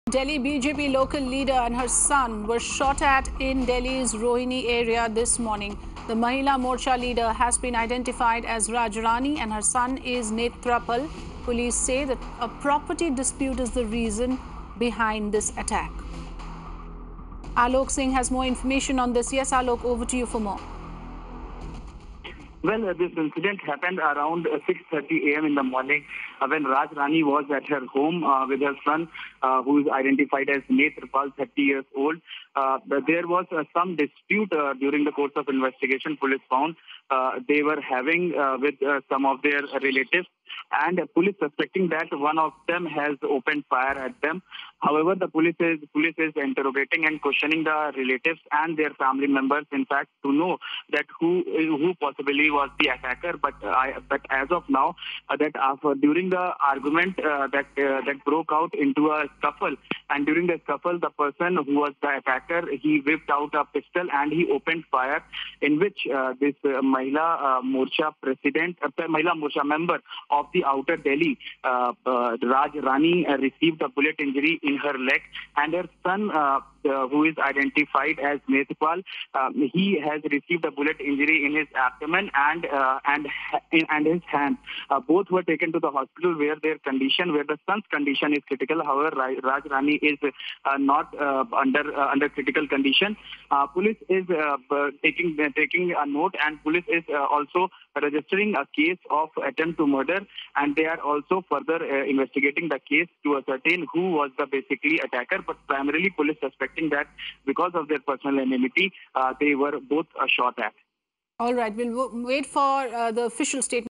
BJP mahila morcha worker & her son shot at in Delhi's Rohini, Police investigation underway
Delhi BJP local leader and her son were shot at in Delhi's Rohini area this morning. According to the police, a property dispute is the reason behind this attack. Watch to know more.